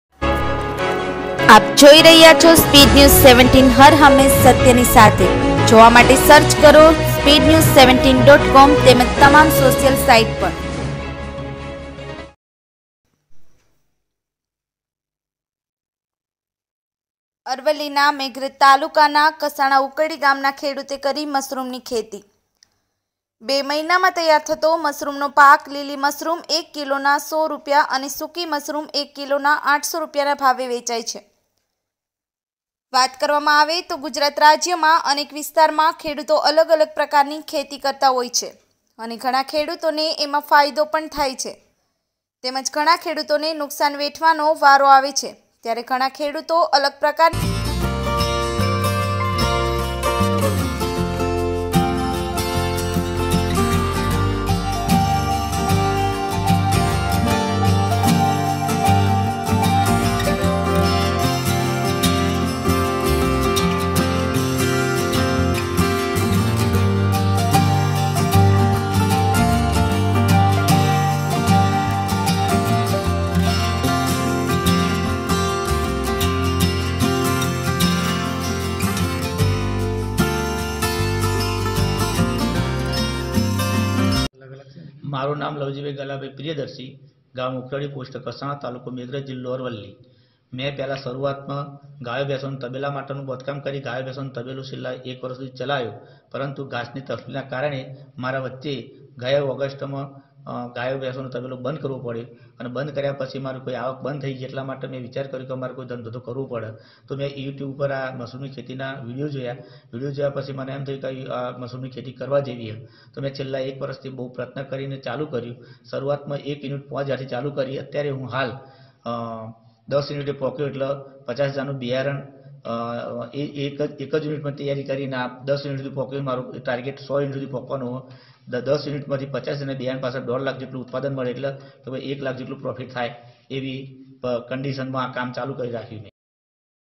ARAVALLI ના મેઘરજ તાલુકાના કસાણા ઉકરડી ગામના ખેડૂતે કરી મશરૂમની ખેતી
અરવલ્લીના મેઘરજ તાલુકાના  કસાણા ઉકરડી ગામના ખેડૂતે કરી મશરૂમની ખેતી

બે મહિનામાં તૈયાર થતો મશરૂમનો પાક લીલી મશરૂમ એક કિલોના 100 રૂપિયા અને સૂકી મશરૂમ એક કિલોના 800 રૂપિયાના ભાવે વેચાય છે

વાત કરવામાં આવે તો ગુજરાત રાજ્યમાં અનેક વિસ્તારમાં ખેડૂતો અલગ અલગ પ્રકાર ની ખેતી કરતા હોય છે અને ગણા ખેડૂતો ને એમાં ફાયદો પણ થાય છે તેમજ ગણા ખેડૂતો ને નુકશાન વેઠવાનો વારો આવે છે ત્યારે ઘણા ખેડૂતો અલગ પ્રકારની ખેતી તરફ વર્યા છે જેમાં વાત કરીયે તો મેઘરજ તાલુકાના કસાણા ઉકરડી ગામના એક ખેડૂતે પોતે ખેતીમાં નવીન ક્રાંતિ લાવવા અને મજૂરી થી બચવા માટે ફક્ત બે મહિનામાં તૈયાર થતી મશરૂમ ની ખેતી કરી અનેક ખેડૂતો ને અગાવું ઉદાહરણ પુરું પાડવામાં આવ્યું છે મશરૂમ ની ખેતી એક એવી ખેતી છે કે જેને પ્લાસ્ટિક ની થેલી માં ઘાસ ની અંદર મશરૂમ મુકવામાં આવે છે અને આ મશરૂમ ને ઠંડી જગ્યામાં રાખવામાં આવેછે તેમજ આ એક યુનિટ પાછળ આશરે પાંચ હજાર નો ખર્ચો થતો હોય છે ત્યારે ઉકરડી ગામના ખેડૂતે દસ યુનિટ તૈયાર કર્યા છે જેમાં આશરે પચાસ હજાર નો ખર્ચો થયો છે તેમજ આ મશરૂમ નો પાક બે મહિના પછી તૈયાર થયીને બીજા રાજ્યમાં મોકલવામાં આવે છે જેમાં લીલી મશરૂમ નો ભાવ કિલોના 100 રૂપિયા અને સૂકી મશરૂમ ના 800 રૂપિયાના ભાવે વેચી શકાય છે ત્યારે આવા સમયે કસાણા ઉકરડીના ખેડૂતે મશરૂમની ખેતી કરી ખેતીના ખર્ચા કરતા ત્રણ ગણા રકમની આવક મેળવી નવીન ખેતી તરફ પ્રયાસ કર્યો છે ત્યારે આ ખેતી દ્વારા સમાજ તેમજ ખેડૂતોને અગાવું ઉદાહરણ પુરું પાડ્યું છે

મશરૂમ ની ખેતી કઈ રીતે થાય...

મશરૂમ એ એક આરોગ્યપ્રદ ખોરાક છે જે ઝડપથી વિકસતી બેસીડીઓમાય સેટસ નામની એક ફૂગ છે જે માંસલ ફળના શરીર નું ઉત્પાદન કરે છે તેમાં પ્રોટીન, વિટામિન, ખનીજો થી સમૃદ્ધ હોય છે તેથી તેને ઉર્જા સમૃદ્ધ ખોરાક તરીકે પીવામાં આવે છે

રિપોર્ટર: અનીસ આદમ મોડાસા અરવલ્લી